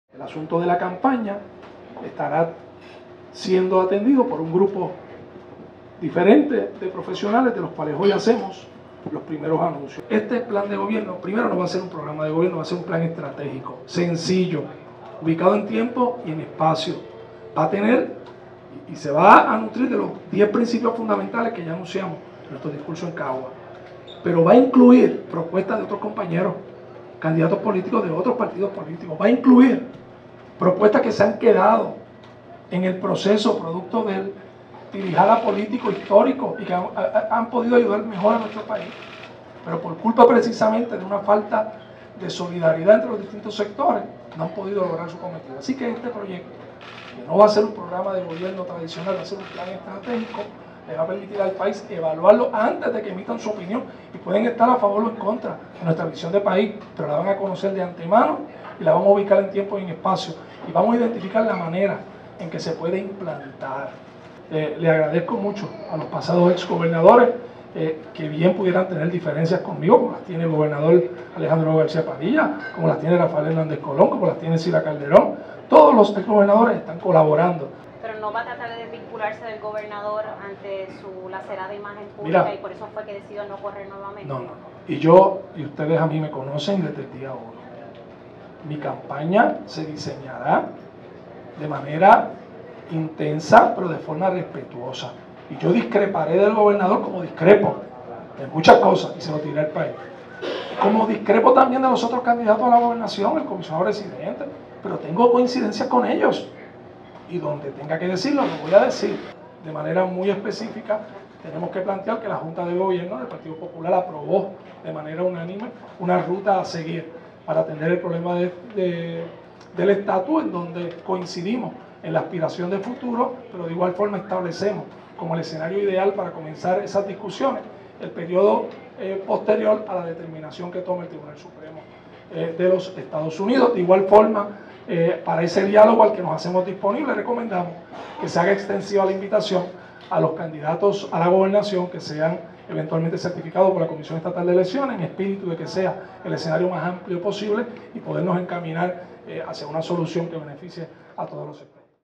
David Bernier hace énfasis en la unidad de propósito
Durante la presentación de nombramientos para su equipo de campaña, el candidato a la gobernación por el Partido Popular Democrático (PPD), sostuvo su postura de convocar la inclusión de todos los partidos a la discusión de temas neurálgicos como el status político.